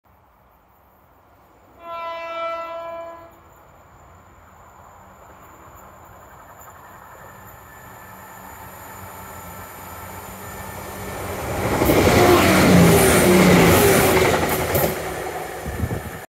ScotRail 170404 on 1T99 Inverness - Glasgow Queen Street
ScotRail DMU 170404 glides down the Highland Mainline near Murthly whilst working 1T99 Inverness - Glasgow Queen Street.